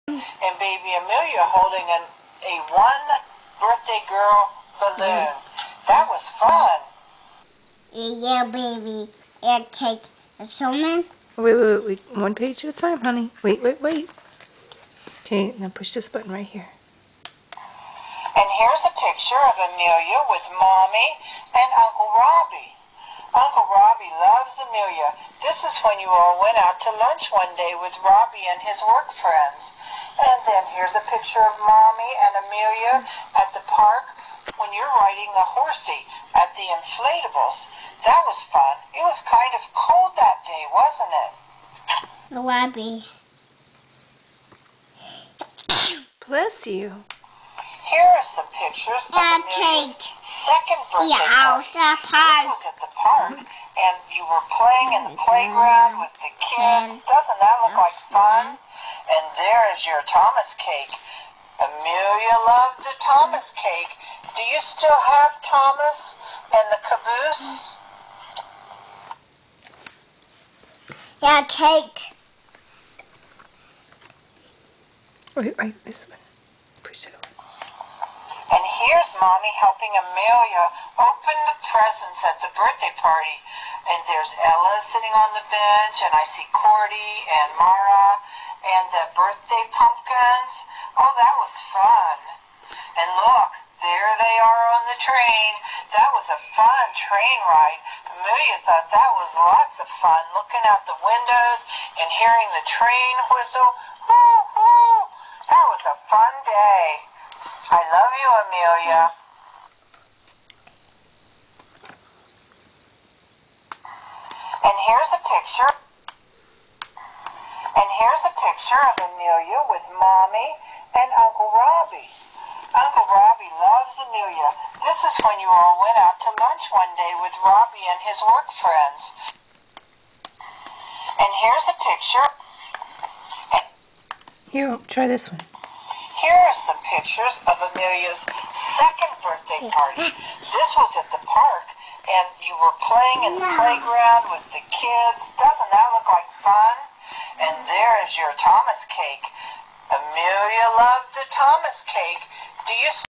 Amelia listens to Gamma Kern's talking photo album
Amelia's 2nd Christmas